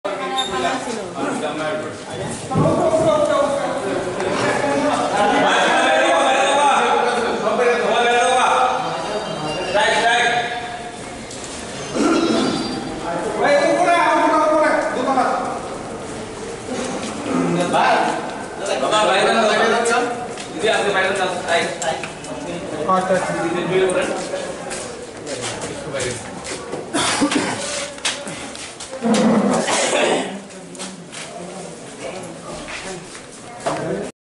Congress's picketing during Tripura Bandh in Govt offices today.
Strike in Protest of Police's attack on Journalists, Activists : Congress's picketing during Tripura Bandh in Govt offices today.